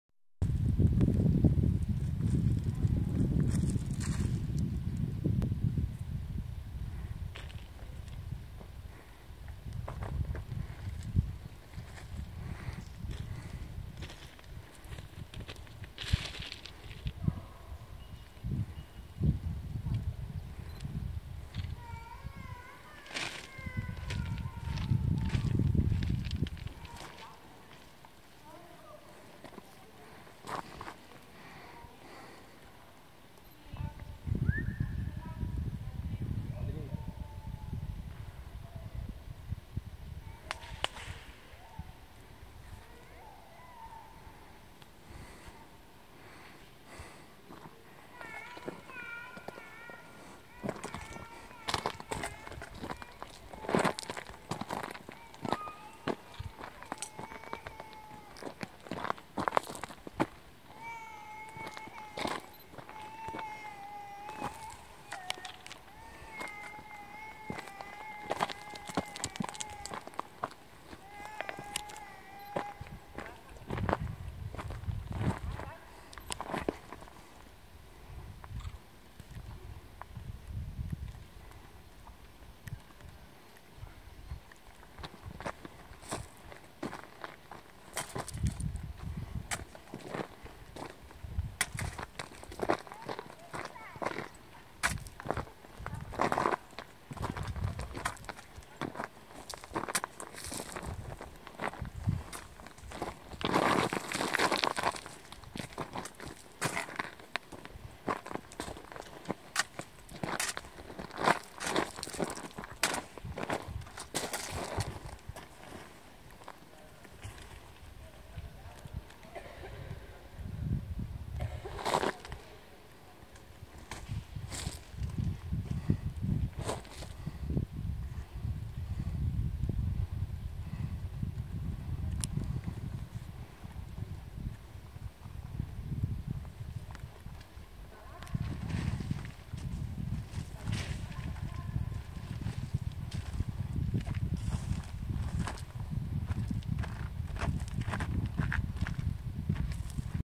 MAJELLA Madre .... le gole di San Martino
Breve panoramica delle Gole di San Martino e dei ruderi del Monastero Benedettino.